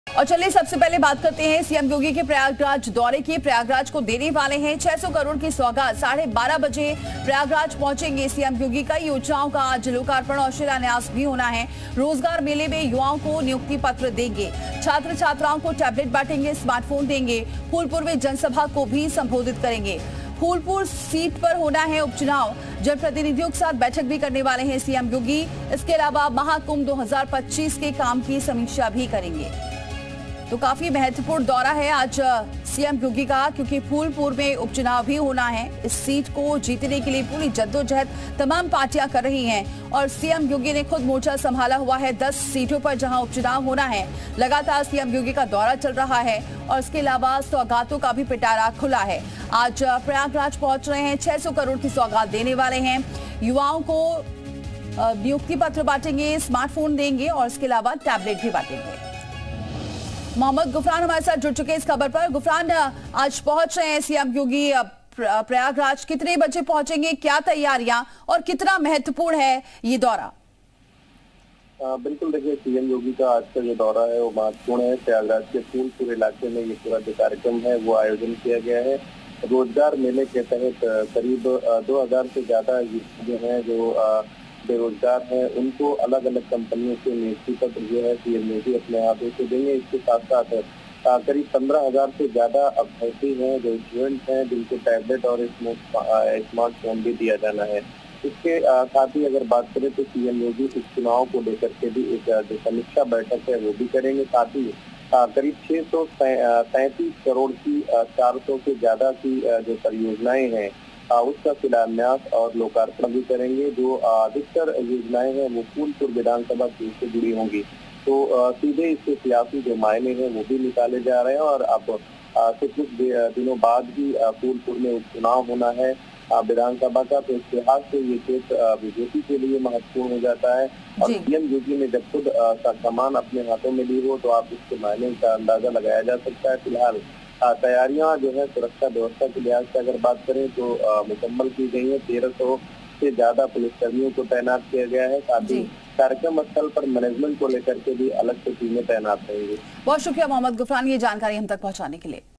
CM Yogi Prayagraj Visit: CM Yogi का आज Prayagraj दौरा,Prayagraj को देंगे 600 करोड़ की सौगात Breaking
CM Yogi Prayagraj Visit : सीएम योगी का आज प्रयागराज दौरा, प्रयागराज को देंगे 600 करोड़ की सौगात, कई योजनाओं का करेंगे लोकार्पण और शिलान्यास, रोजगार मेले में युवाओं को देंगे नियुक्ति पत्र

CM Yogi Prayagraj Visit: CM Yogi will visit Prayagraj today, will give a gift of Rs 600 crore to Prayagraj, will inaugurate and lay the foundation stone of many schemes, will give appointment letters to the youth in the employment fair.